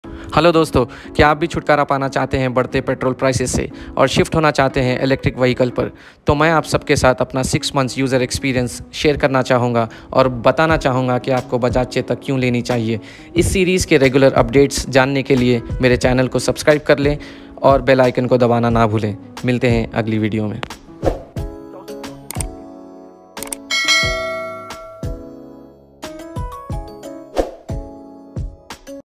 Bajaj Chetak EV Electric Scooter | Part 1
Bajaj Chetak EV 6 Months User Experience | Chetak Mileage Range Price Booking Detail